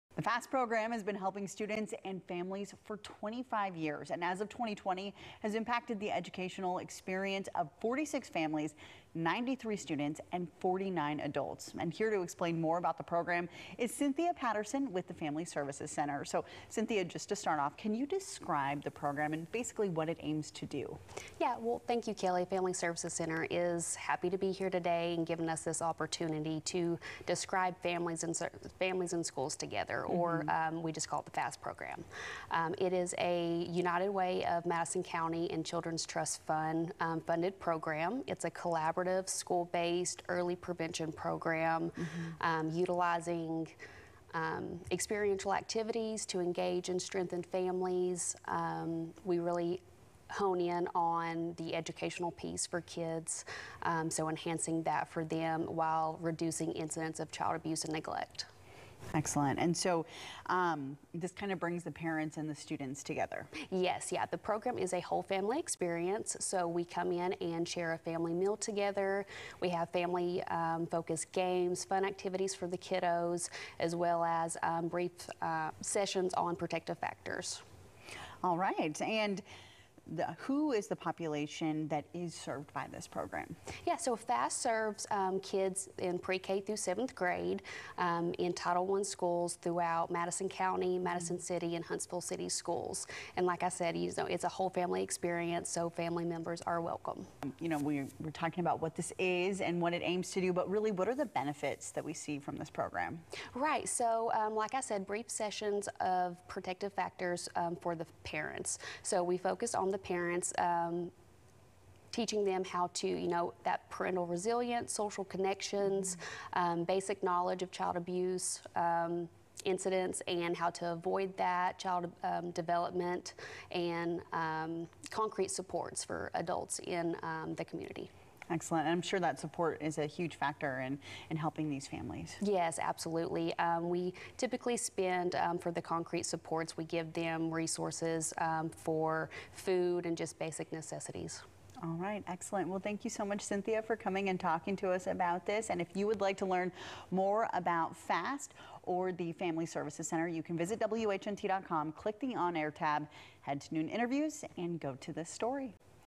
'FAST' Marks 25 Years Of Helping Students And Families
As the school year starts, so does a program from the Family Services Center that brings students, parents, and services together in one place.